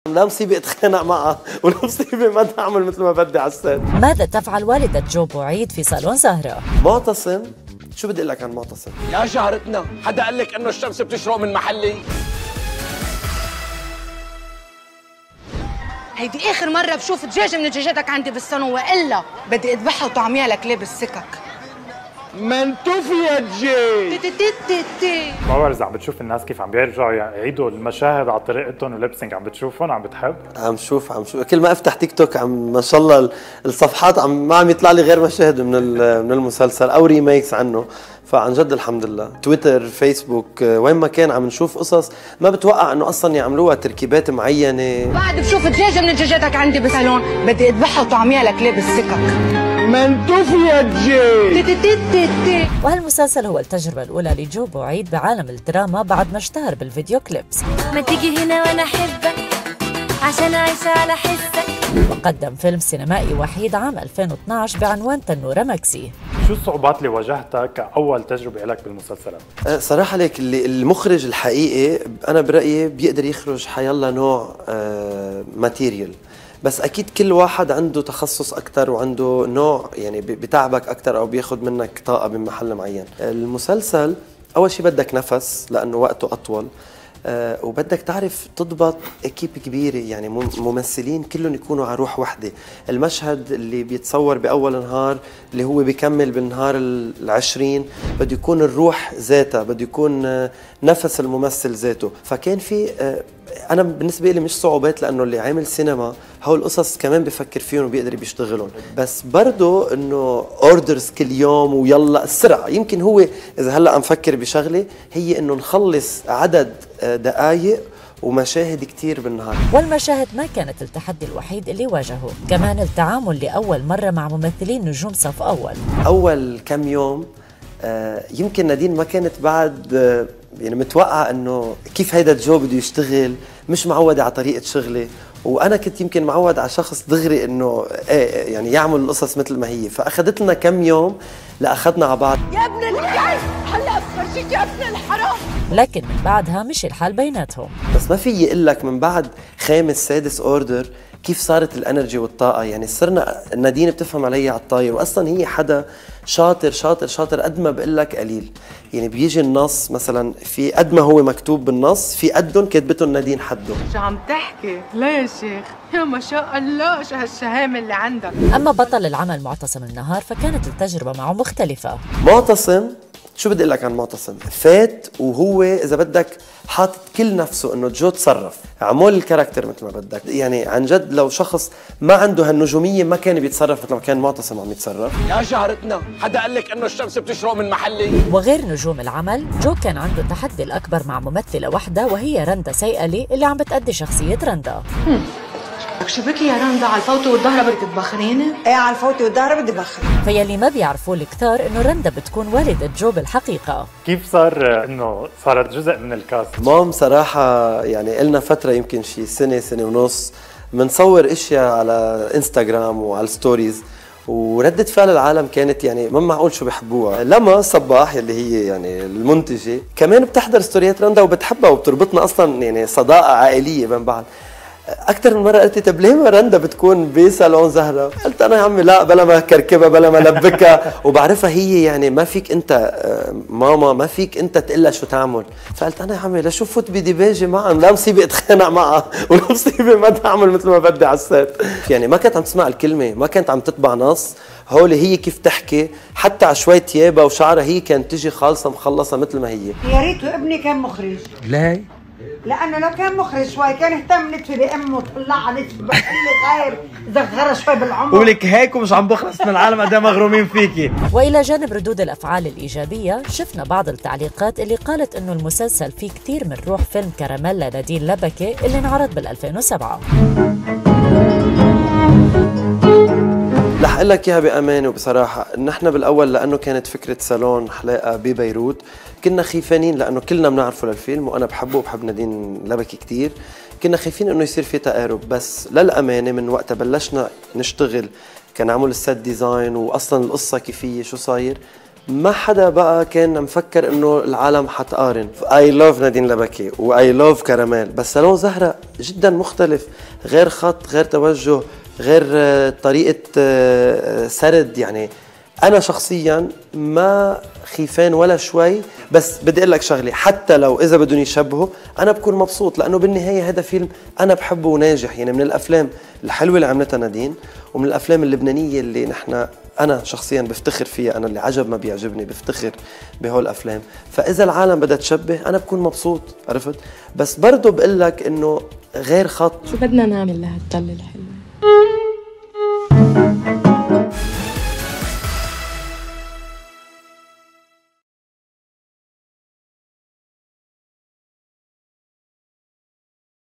💬ماذا تفعل والدة جو بو عيد في " صالون زهرة" ؟
💬"كل ما أفتح تيك توك ما شاء الله الصفحات ما عم يطلعلي غير مشاهد من المسلسل أو Remakes” هكذا علّق جو بو عيد على أصداء مسلسل صالون_زهرة والذي يعد التجربة الأولى لجو بو عيد بعالم الدراما بعد ما اشتهر بالـفيديو كليب وقدم فيلم سينمائي وحيد عام 2012 بعنوان "تنورة مكسي" .
جو حدثنا في هذا اللقاء عن الصعوبات والتحدي الذي واجهه بالإضافة إلى تجربة التعامل لأول مرة مع ممثلين نجوم صف أول. ويبدو أن التجربة مع بطل العمل معتصم النهار كانت مختلفة! لكن السؤال هنا، ماذا تفعل والدة جو بو عيد في " صالون زهرة" ؟